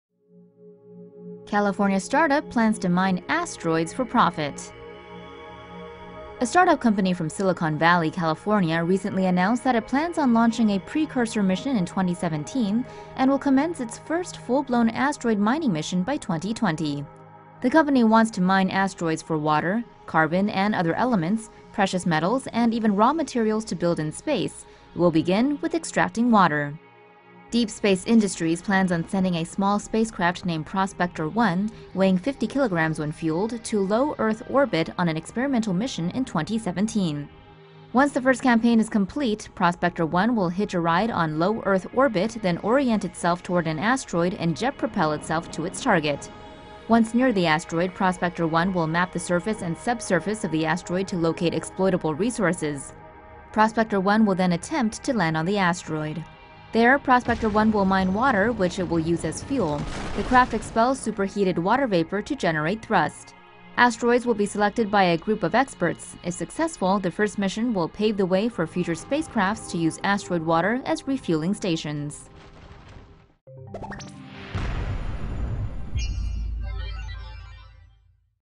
Asteroid mining: Deep Space Industries to harvest asteroids by 2020 - TomoNews
SILICON VALLEY, CALIFORNIA — A California startup plans to mine asteroids for profit.

The company from Silicon Valley, Deep Space Industries, recently announced that it plans on launching a precursor mission in 2017 and will commence its first mission by 2020. The company wants to mine asteroids for water, carbon and other elements, precious metals, and even in-space building materials. It will begin with extracting water.

Deep Space Industries plans on sending a small spacecraft named Prospector-1 to low-Earth orbit on an experimental mission. The Prospector-1 weighs 50 kilograms when fueled and was designed to be cost effective.

Once the initial campaign is complete, Prospector-1 will hitch a ride on low-Earth orbit then point towards an asteroid and jet propel itself to its target.

Once near, Prospector-1 will map the surface and subsurface of the asteroid locating resources. Prospector-1 will also attempt to land on the asteroid.

On the asteroid, Prospector-1 will mine water, which it will use as fuel. The craft expels superheated water vapor to generate thrust.

Asteroids will be selected by a group of experts. If successful, the first mission will pave way for future spacecrafts to use asteroid water as refueling stations.